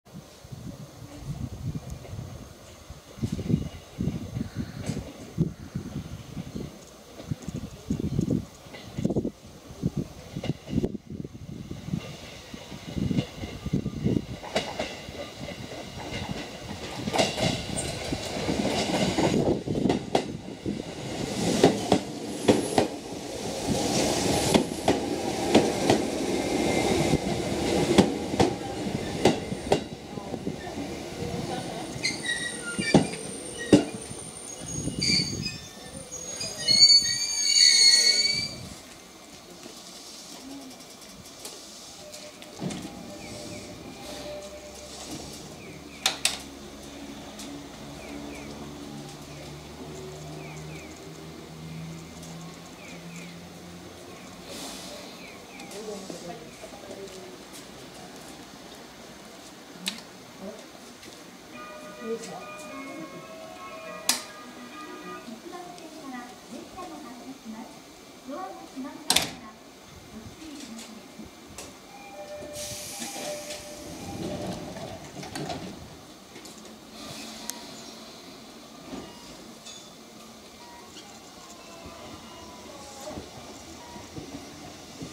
日光線下野大沢駅での車掌動作２
２０２２年２月１６日に撮影しました。続いては宇都宮方面１番線ホームでの車掌動作です。